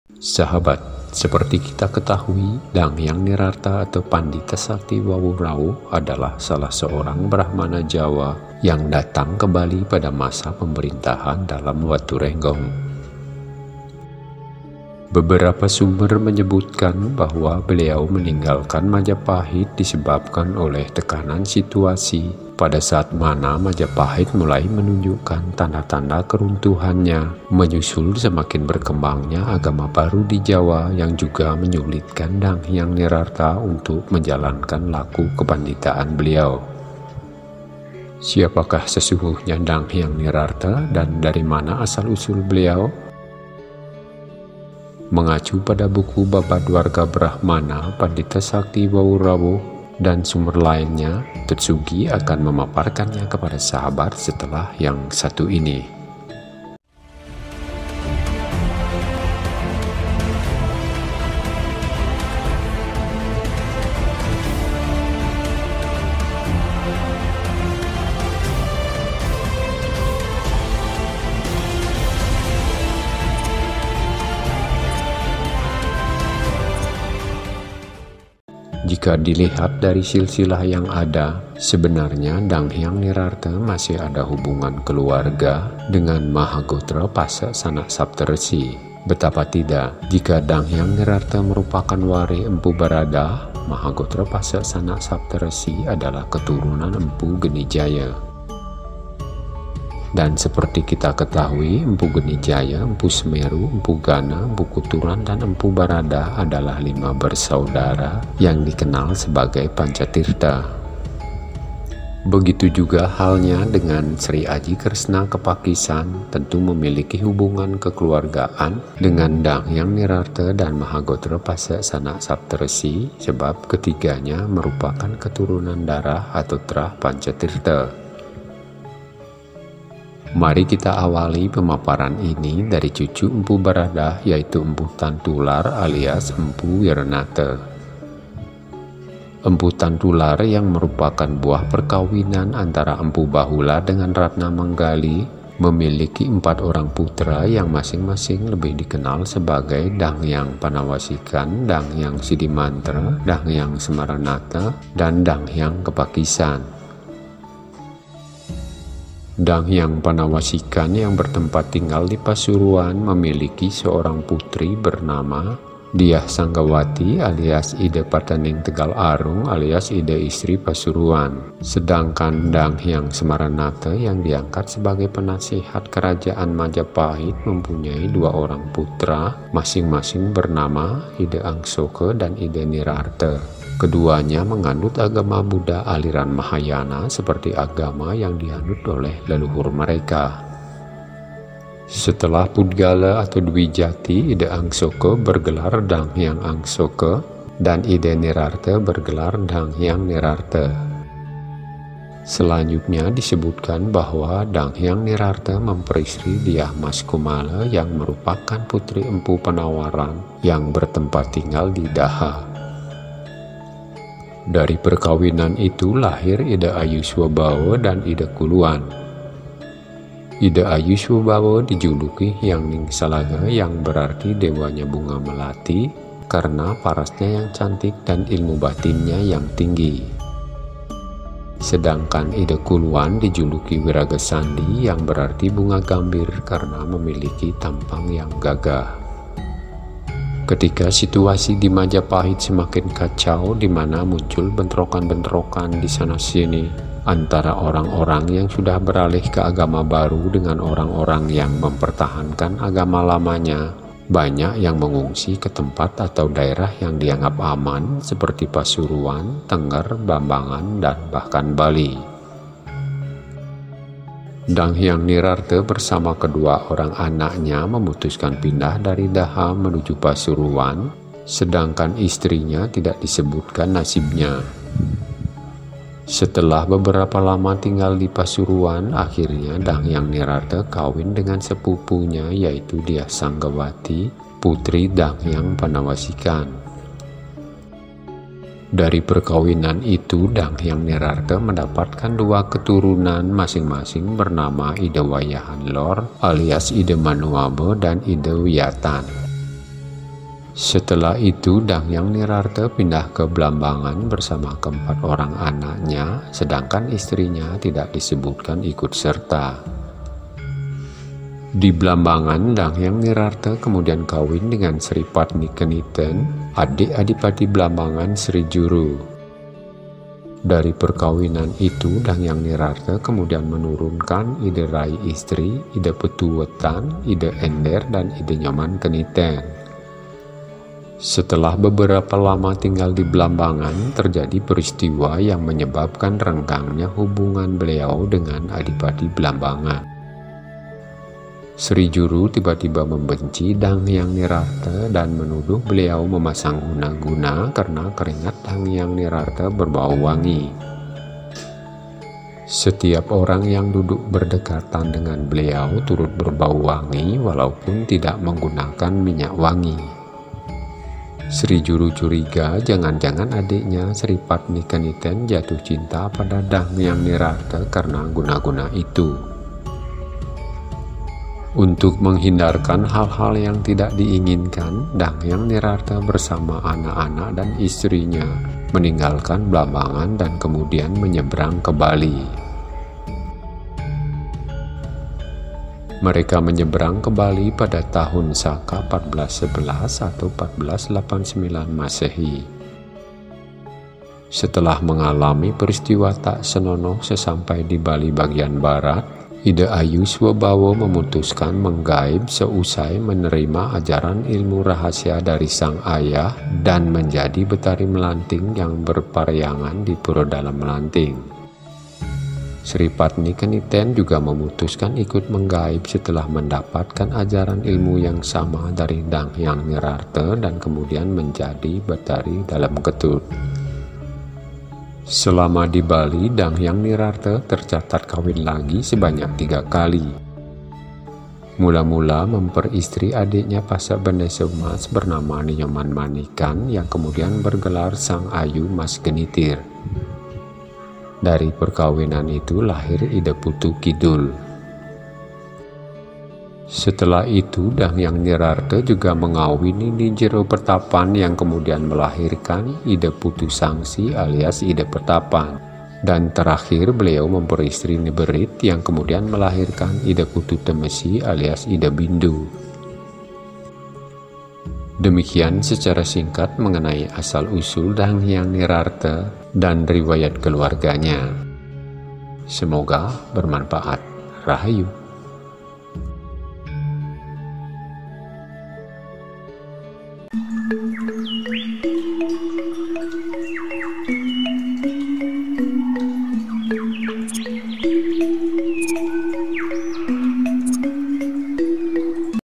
Asal-Usul Danghyang Nirartha dan Riwayat Keluarganya
Jika dilihat dari silsilah yang ada, sebenarnya Danghyang Nirartha masih ada hubungan keluarga dengan Maha Gotra Pasek Sanak Sapta Resi. Betapa tidak. Jika Danghyang Nirartha merupakan warih Mpu Baradah, Maha Gotra Pasek Sanak Sapta Resi adalah keturunan Mpu Gnijaya. Dan seperti kita ketahui, Mpu Gnijaya, Mpu Semeru, Mpu Ghana, Mpu Kuturan dan Mpu Baradah adalah lima bersaudara yang dikenal sebagai Panca Tirta.

Begitu juga halnya dengan Dalem Sri Aji Kresna Kepakisan, tentu memilki hubungan kekeluargaan dengan Danghyang Nirartha dan Maha Gotra Pasek Sanak Sapta Resi, sebab ketiganya merupakan keturunan darah atau trah Panca Tirta